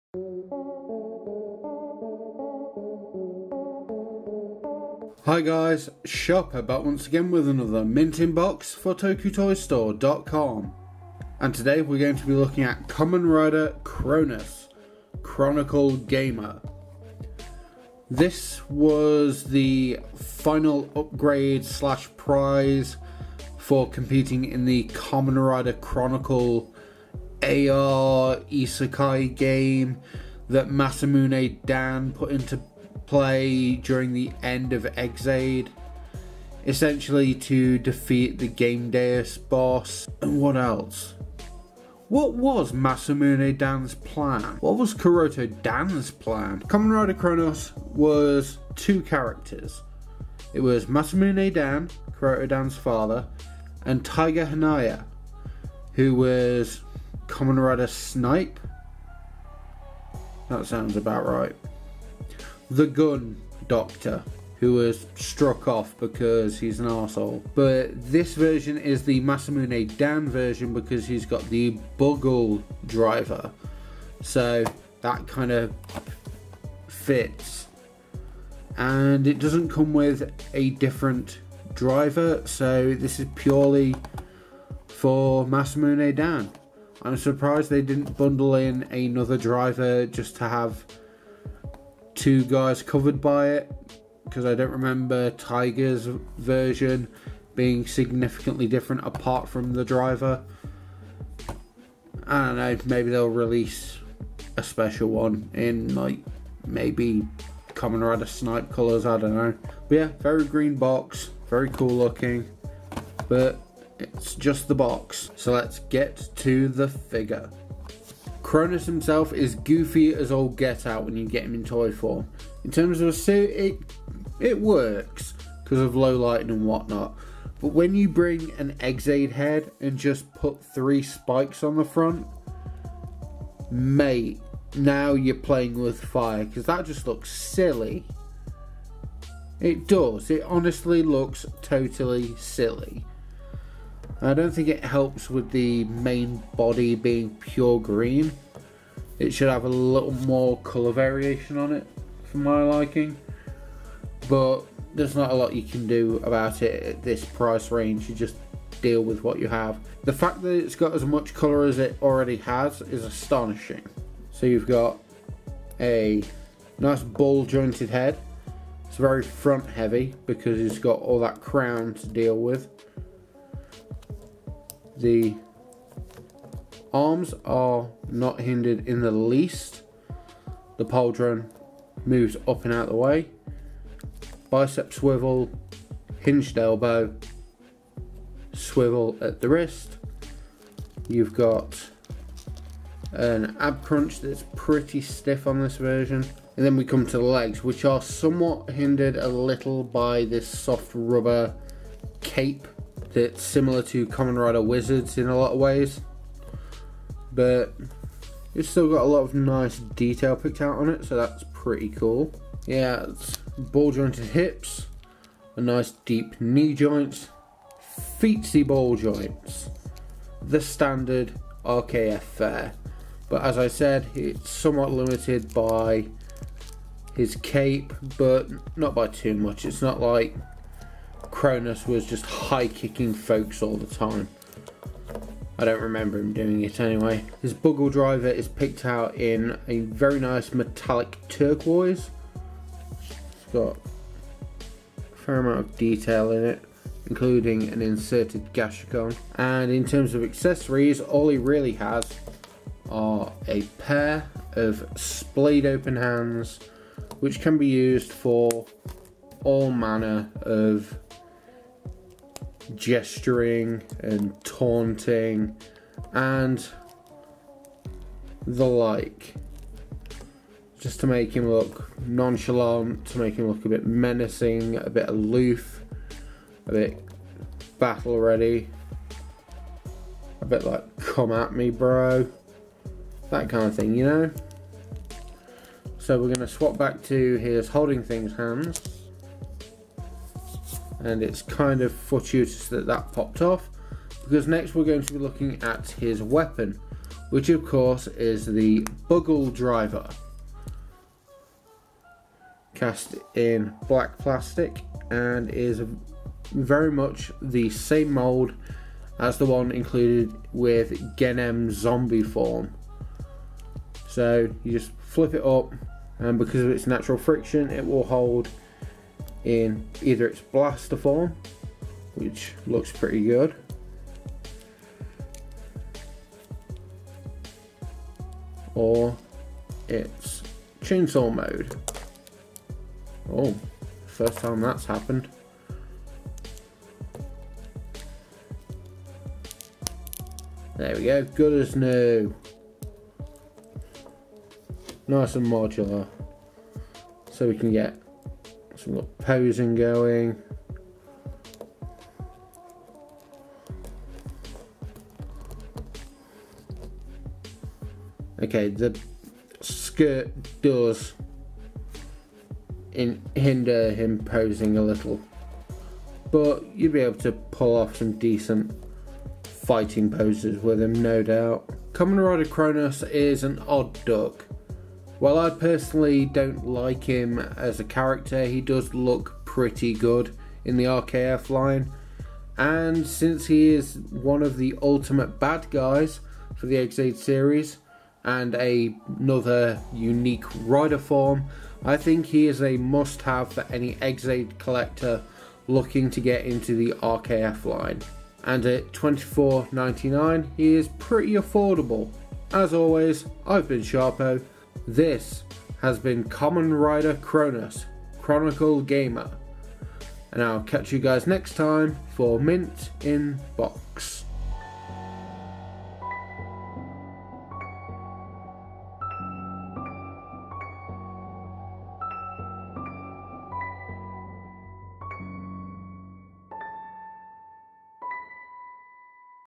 Mint in Box Review | Rider Kicks Figure Series | Kamen Rider Cronus
Sharp-O's back with another Mint in Box for the man who would rule the world, Masamune Dan aka Kamen Rider Cronus. It's Judgement Time.